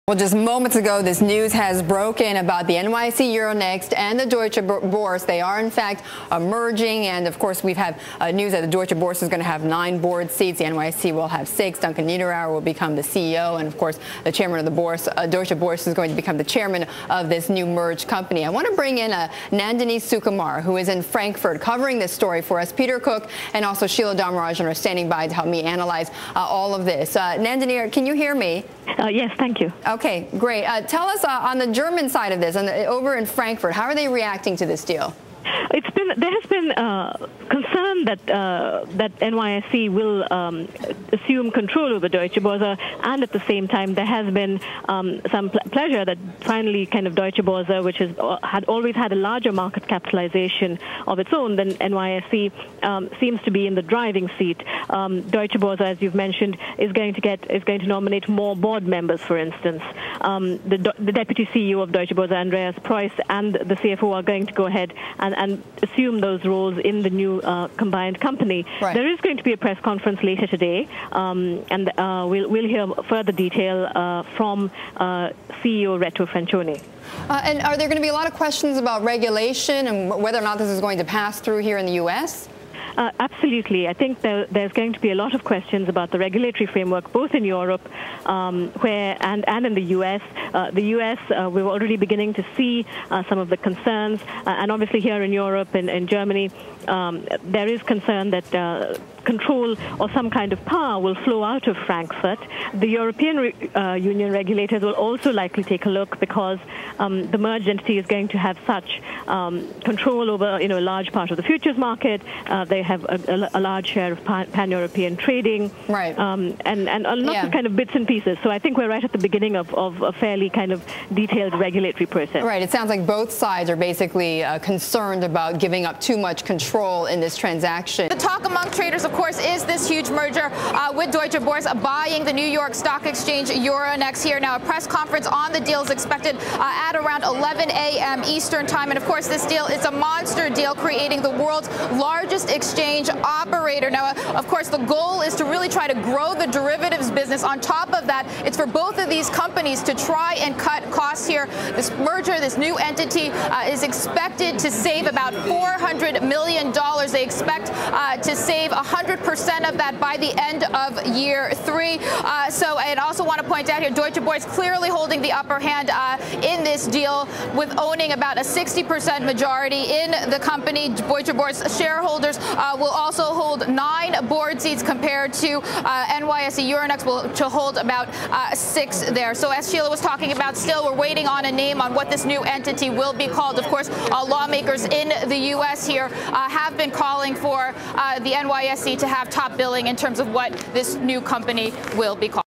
Deutsche Boerse Buys NYSE to Form Top Exchange Owner
Feb. 15 (Bloomberg) -- Deutsche Boerse AG, operator of the Eurex futures platform and Frankfurt Stock Exchange, agreed to buy New York Stock Exchange parent NYSE Euronext for $9.53 billion in a deal that creates the world's largest owner of equities and derivatives markets. Bloomberg's Nandini Sukumar and Laura Lee report. (Source: Bloomberg)